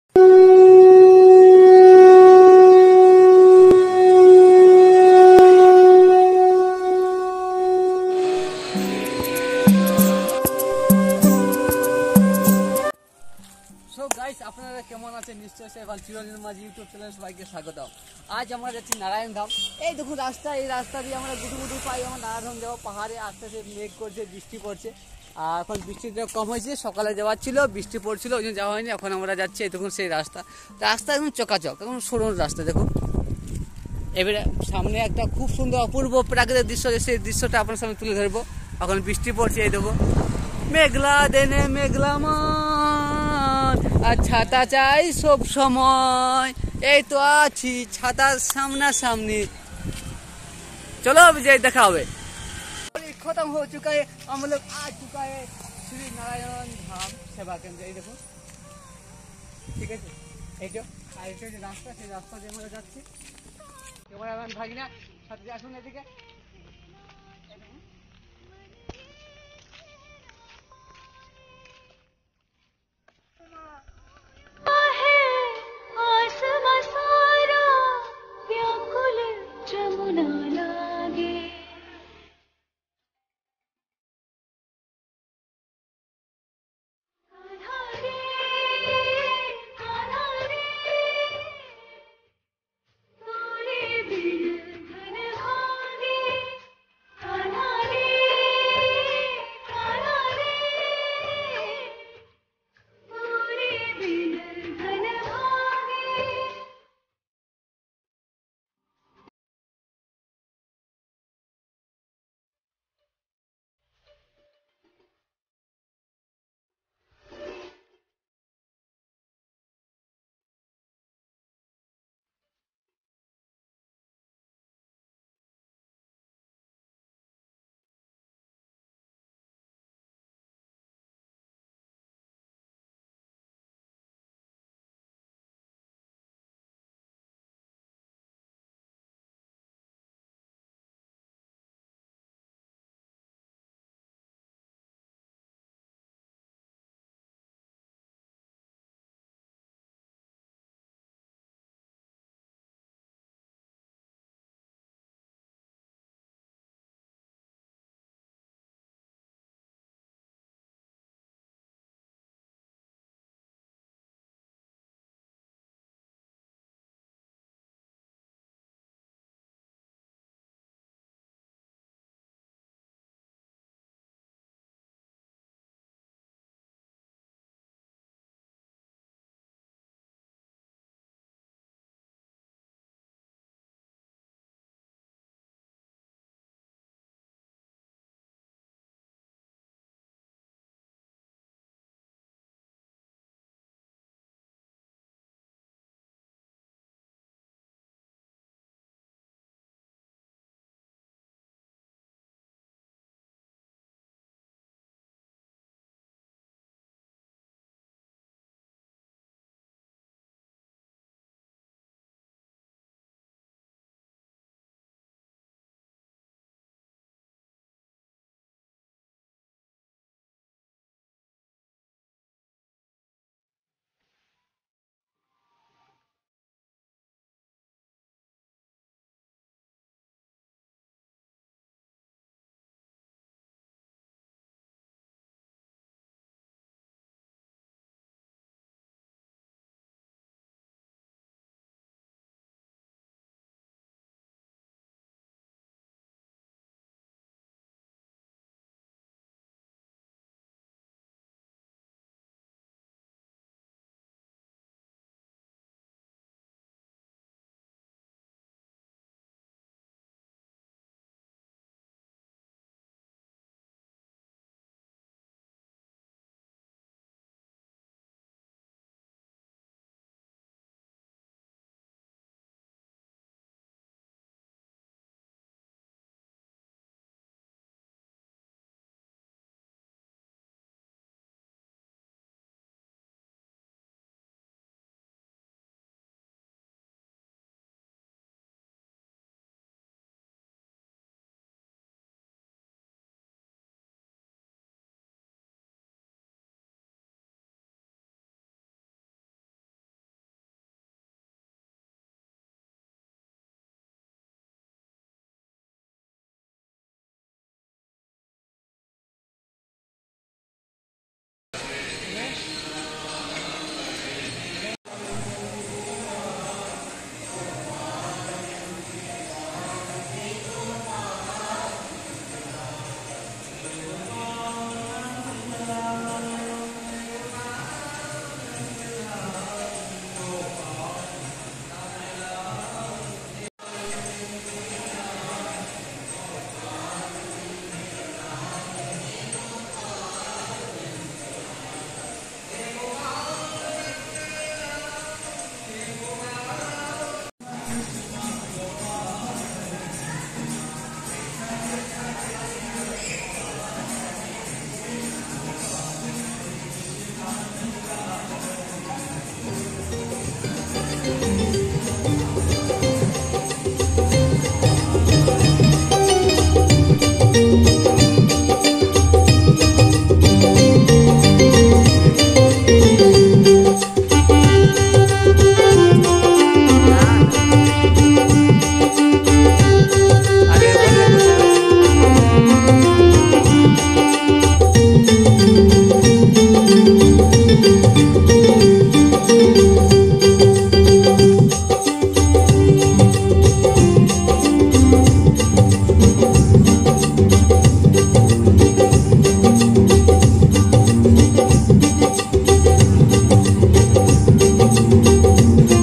Narayan Dham in Lonavla (Pune)
Narayan Dham in Lonavla (Pune) ‎@Chiranjit Majhi Vlog
Narayan Dham trample অবস্থান হচ্ছে পুনে জেলায় মহারাষ্ট্র রাজ্যের।